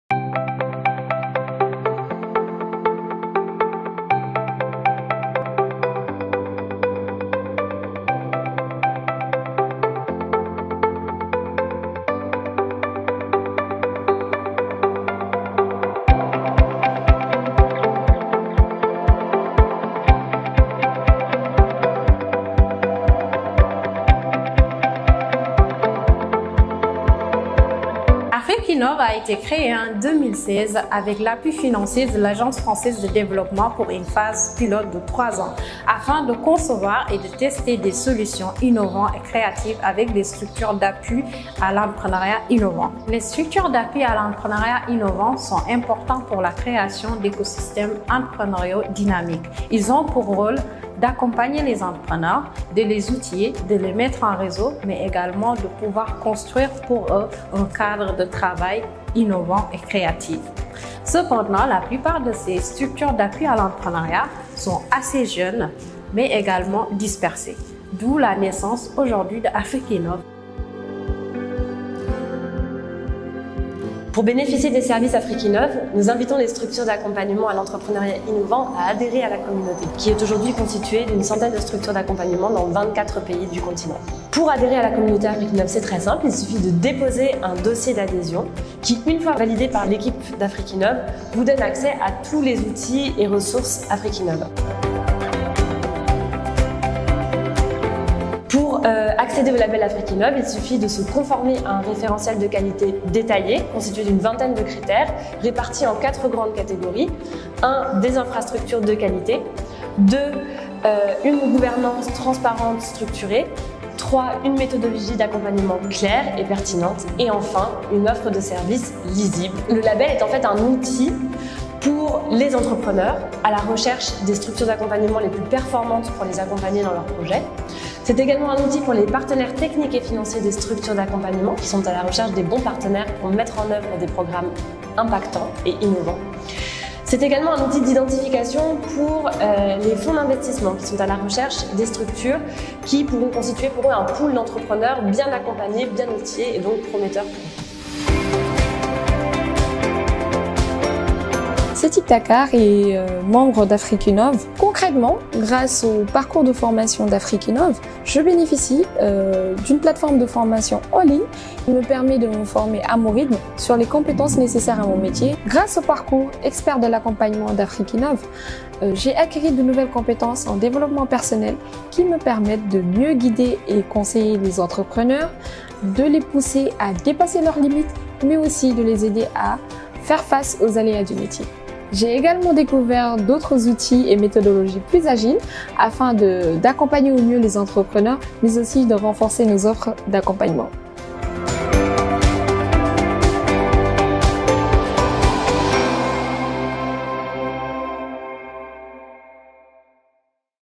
Afric’Innov soutient les structures d’appui à l’entrepreneuriat innovant
Afric’innov, grâce au soutien du Fonds d’amorçage by Digital Africa, déploie un programme de renforcement de capacité spécifiquement étudié pour les structures d’appui à l’entrepreneuriat innovant (SAEI) en Afrique.

Le fonds d’amorçage Digital Africa est dédié au développement d’un entrepreneuriat innovant en Afrique. Doté d’une enveloppe de 15 millions d’euros, il permet à l’AFD de soutenir les programmes de six partenaires locaux qui accompagnent et financent les start-up numériques dès le début de leur activité.